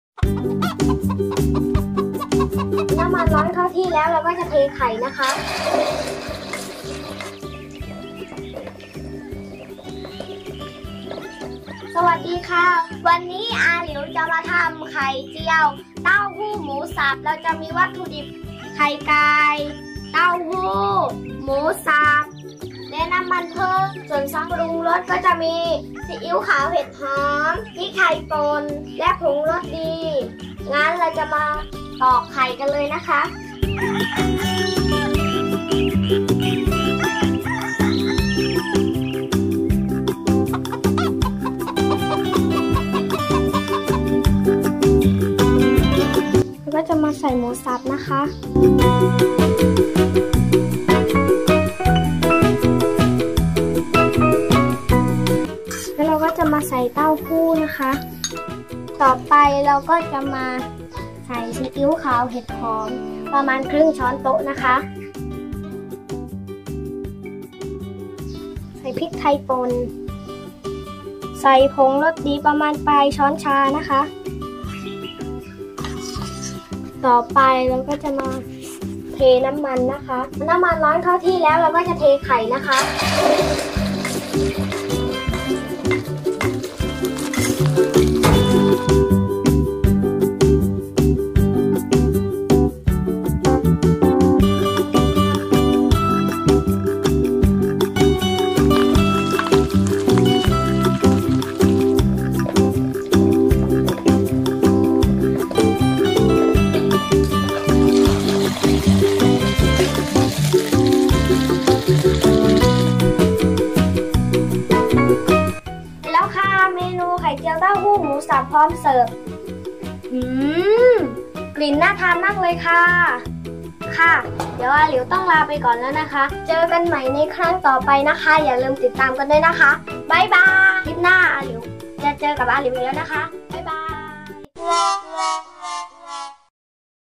แจกสูตร ไข่เเจียวเต้าหู้หมูสับ เมนูไข่ ทำกินเอง Tofu - Eggs with pork chops omelet
วัตถุดิบ คือ
1.ไข่ไก่                                                    1         ฟอง
2.เต้าหู้ไข่ (หั่นเป็นแว่นใหญ่หน่อย)        1       หลอด
3. หมูสับ                                                  1   ช้อนโต๊ะ
4. น้ำมันพืช                                         ใช้ปริมาณพอดี

เครื่องปรุงรส มีดังนี้ คือ
1. ซีอิ๊วขาวเห็ดหอม                    ครึ่งช้อนโต๊ะ
2. พริกไทยป่น                            ปลายช้อนชา
3. ผงรสดี                                    ปลายช้อนชา

ปล.โปรดติดตามผลงานอาหลิวในคลิปต่อๆ ไปด้วยนะคะ
สามารถติดตามเพจได้ที่
R liew Noopin kids

(อาหลิว/หนูปิ่น สร้างความบันเทิงอย่างมีสาระให้แก่เด็กๆ)
R liew Noopin kids พี่น้องพาเพลิน